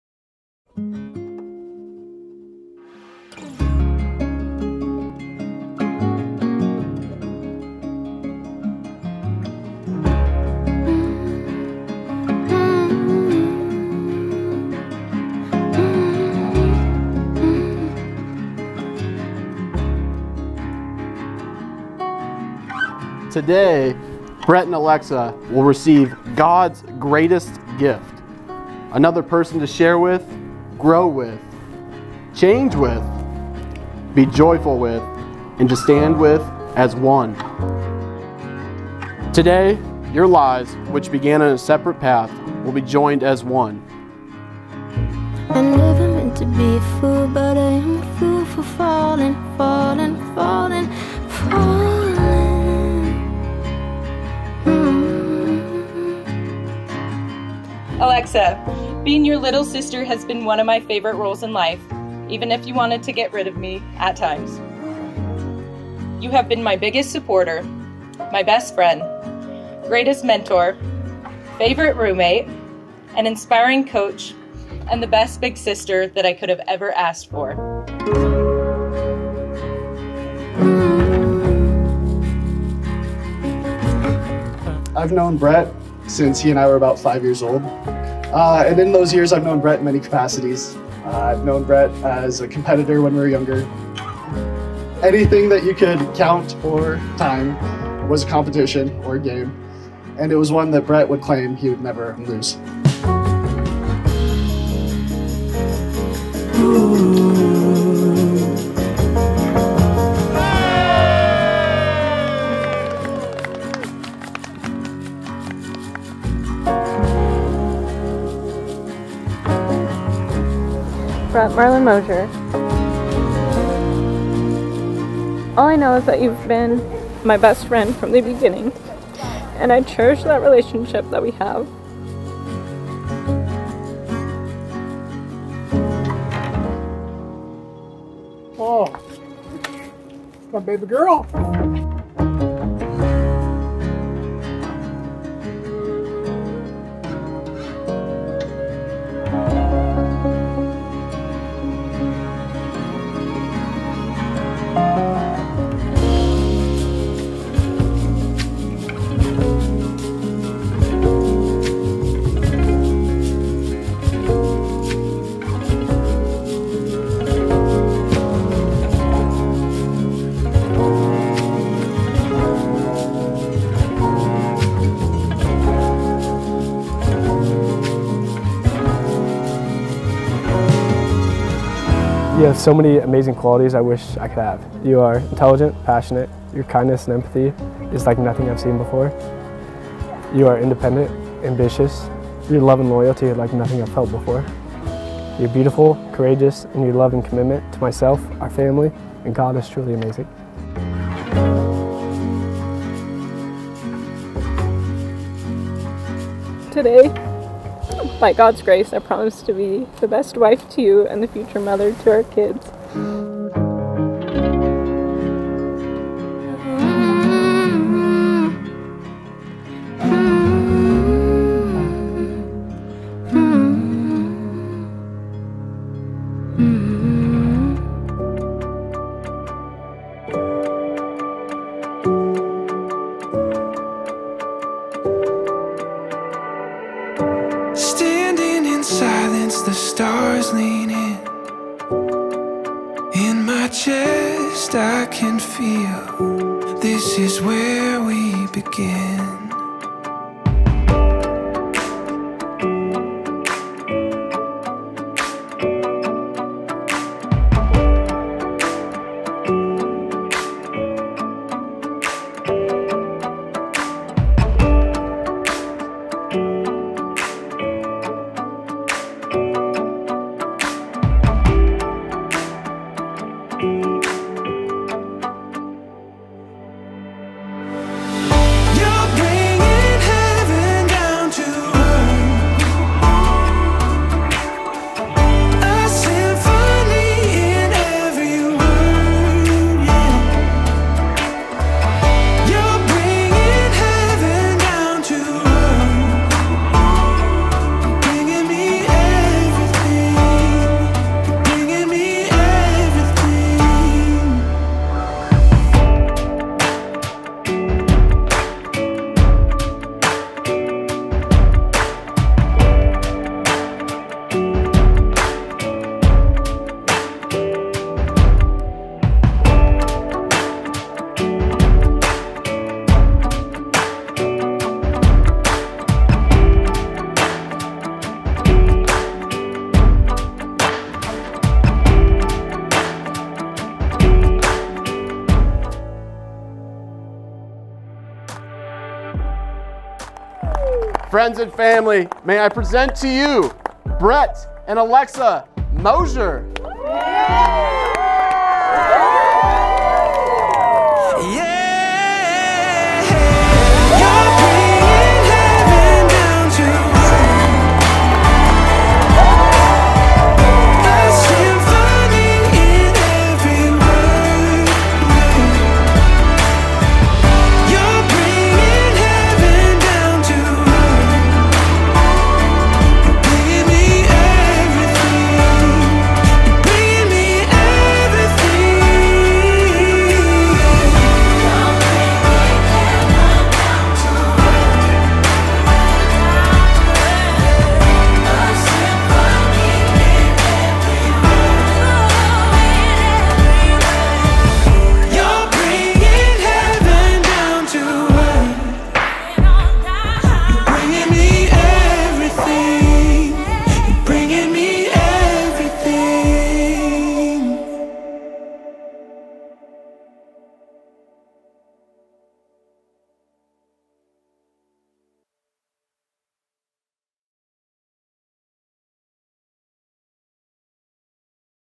Moser Full Wedding Film | Colfax, WA
Brett & Alexa's day was a HOT one out on the Palouse. We had so much fun on their wedding day with the bridal party, family and guests. I love hearing my couples love stories, and these two may have had some of the cutest vows to each other!